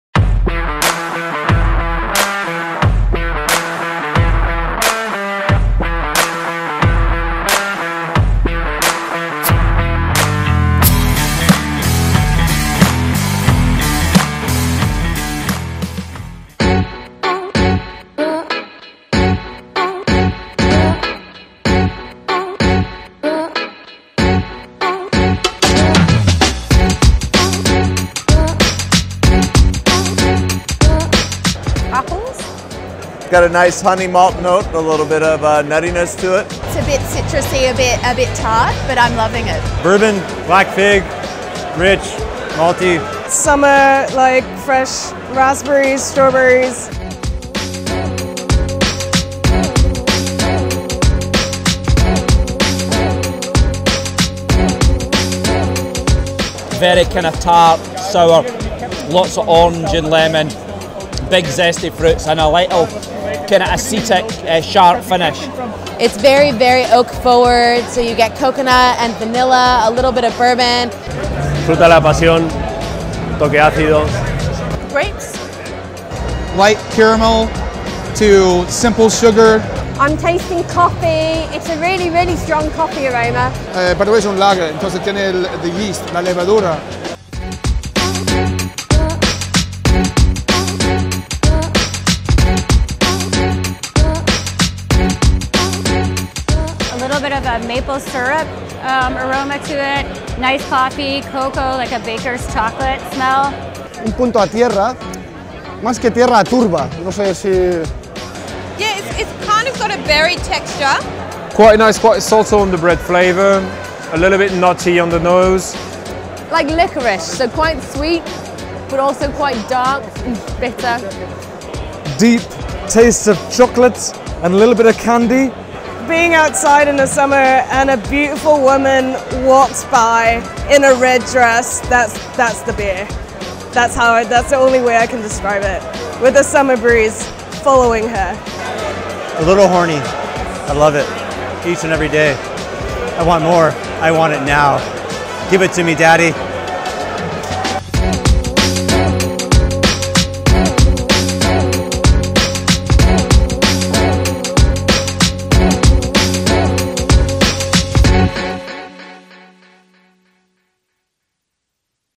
Beer taste´s challenge (Beavertown Extravaganza 2018)
Los pasados 7 y 8 de septiembre, se celebró en Londres (Inglaterra) el festival "Extravaganza18" organizado por la cervecería Beavertown.

Pasada la polémica por la compra de participaciones en la empresa por parte del grupo Heineken, y de la consecuente renuncia a participar en el festival de muchas cerveceras invitadas, este se realizó con aparente normalidad.

Unas 60 cervecerías de todo el mundo se concentraron en el "Pipeworks" londinense para mostrar sus cervezas más enfocadas a las tendencias actuales.

Disfrutamos como niños con tanta variedad e hicimos varias entrevistas a los asistentes preguntando sobre los olores y sabores de la cerveza que estaban tomando en aquel momento. Recopilamos los mejores e hicimos este vídeo.

Esperamos que lo disfrutéis.

Realización y edición: Daniel Ruiz
Agradecimientos: Santi Martín y Albert Navarro